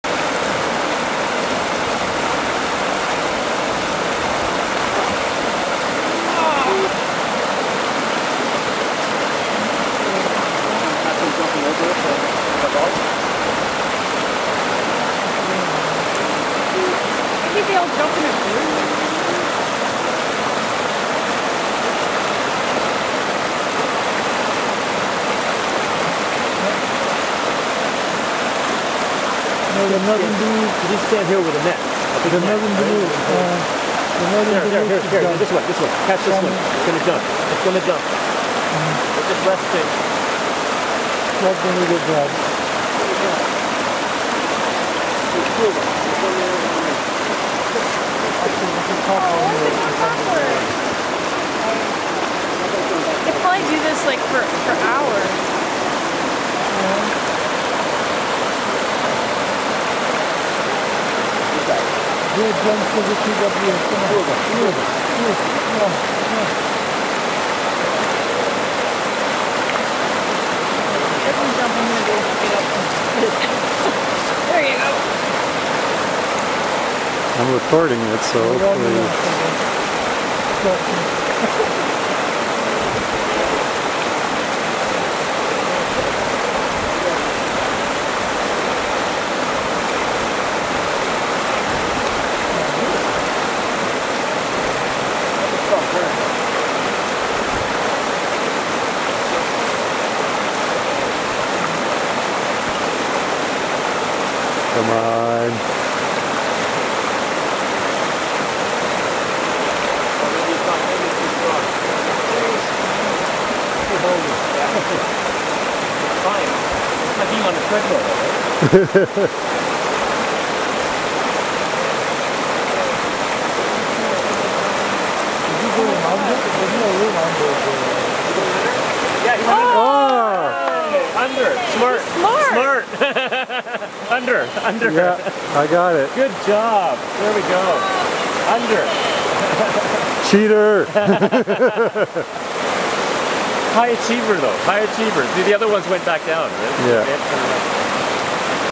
Waiting for a salmon (or trout) to jump over a fallen tree trunk
October 22th, 2011.  This video was taken by Grindstone Creek.  We couldn't figure out if it was a salmon or a trout.  What do you think?